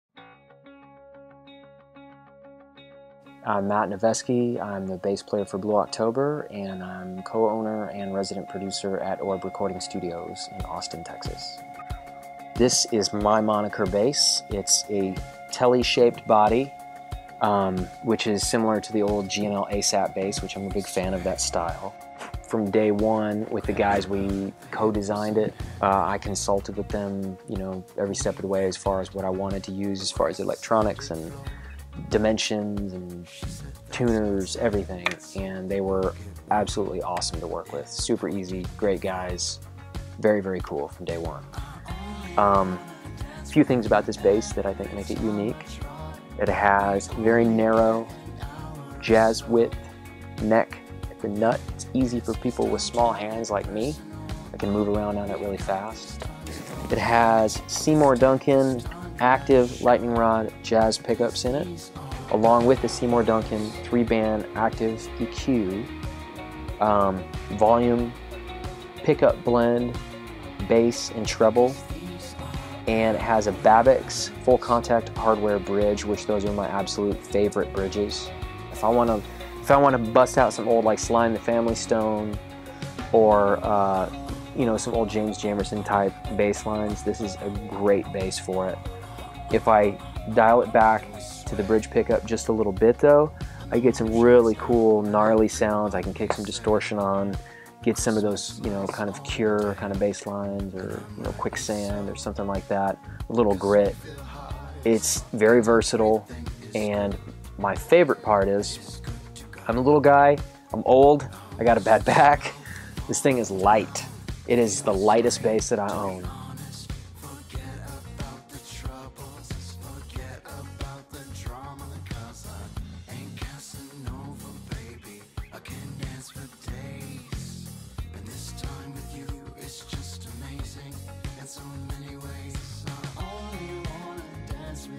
Matt Noveskey of Blue October and Orb Studios on Moniker Guitars' Dixie Bass
We synched up with producer and bassist Matt Noveskey to develop the first Moniker bass guitar. The chambered body is based on the Dixie (a T-style shape). Matt choose to outfit his bass with a Babicz Full Contact bridge, Seymour Duncan pickups and Hip Shot tuners.